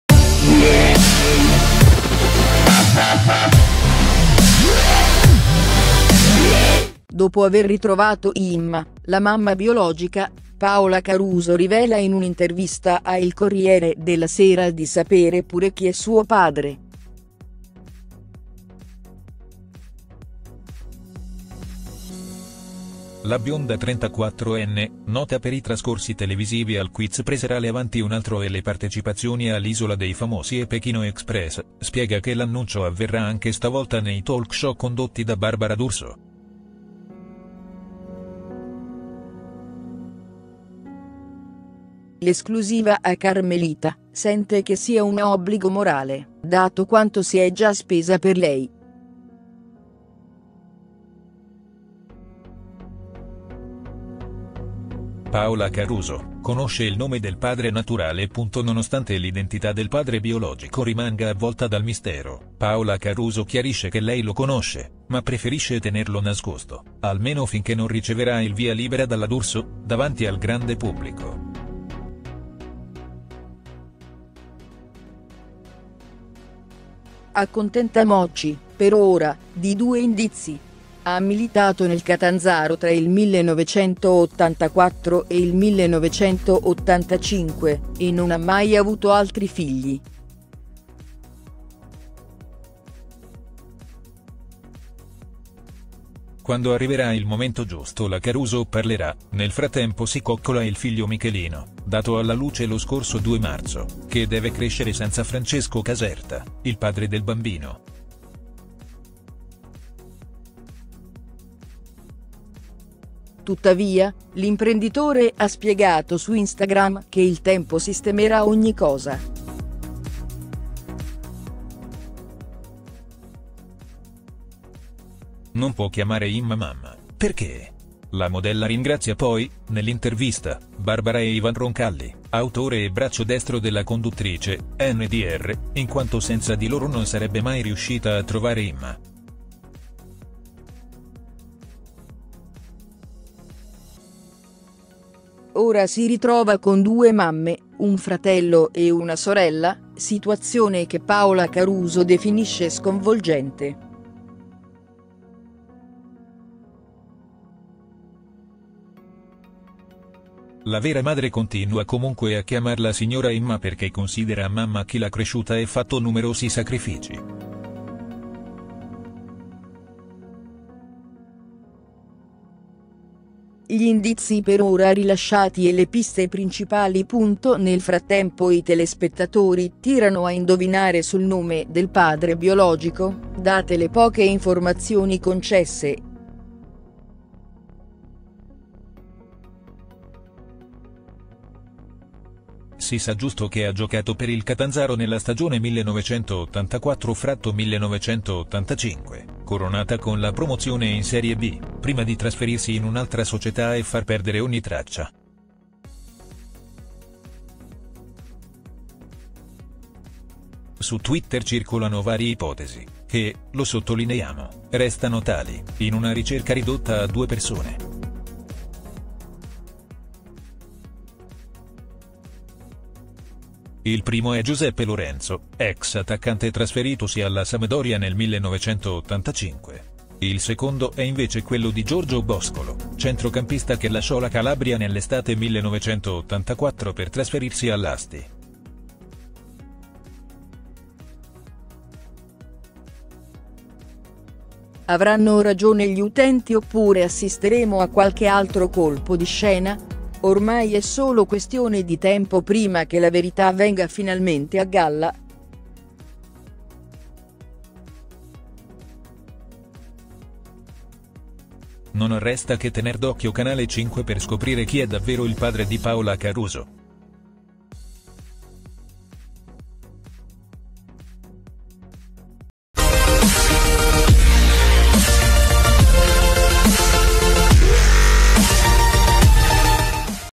Paola Caruso: “So chi è mio padre”, svelato il nome?
Dopo aver ritrovato Imma, la mamma biologica, Paola Caruso rivela in un’intervista a Il Corriere della Sera di sapere pure chi è suo padre. La bionda 34enne, nota per i trascorsi televisivi al quiz preserale Avanti un Altro e le partecipazioni a L’Isola dei Famosi e Pechino Express, spiega che l’annuncio avverrà anche stavolta nei talk show condotti da Barbara D’Urso. L’esclusiva a Carmelita sente che sia un obbligo morale, dato quanto si è già spesa per lei.

Paola Caruso: conosce il nome del padre naturale
Nonostante l’identità del padre biologico rimanga avvolta dal mistero, Paola Caruso chiarisce che lei lo conosce, ma preferisce tenerlo nascosto, almeno finché non riceverà il via libera dalla D’Urso, davanti al grande pubblico. Accontentamoci, per ora, di due indizi: ha militato nel Catanzaro tra il 1984 e il 1985 e non ha mai avuto altri figli.

Quando arriverà il momento giusto la Caruso parlerà, nel frattempo si coccola il figlio Michelino, dato alla luce lo scorso 2 marzo, che deve crescere senza Francesco Caserta, il padre del bambino. Tuttavia, l’imprenditore ha spiegato su Instagram che il tempo sistemerà ogni cosa.

Non può chiamare Imma mamma, perchè?
La modella ringrazia poi, nell’intervista, Barbara e Ivan Roncalli (autore e braccio destro della conduttrice, ndr), in quanto senza di loro non sarebbe mai riuscita a trovare Imma. Ora si ritrova con due mamme, un fratello e una sorella, situazione che Paola Caruso definisce sconvolgente. La vera madre continua comunque a chiamarla signora Imma perché considera mamma chi l’ha cresciuta e fatto numerosi sacrifici.

Gli indizi per ora rilasciati e le piste principali
Nel frattempo i telespettatori tirano a indovinare sul nome del padre biologico, date le poche informazioni concesse. Si sa giusto che ha giocato per il Catanzaro nella stagione 1984/1985, coronata con la promozione in Serie B, prima di trasferirsi in un’altra società e far perdere ogni traccia.

Su Twitter circolano vari ipotesi – che, lo sottolineiamo, restano tali – in una ricerca ridotta a due persone. Il primo è Giuseppe Lorenzo, ex attaccante trasferitosi alla Sampdoria nel 1985. Il secondo è invece quello di Giorgio Boscolo, centrocampista che lasciò la Calabria nell’estate 1984 per trasferirsi all’Asti.

Avranno ragione gli utenti oppure assisteremo a qualche altro colpo di scena? Ormai è solo questione di tempo prima che la verità venga finalmente a galla. Non resta che tener d’occhio Canale 5 per scoprire chi è davvero il padre di Paola Caruso. Gossip, # LaVita24h,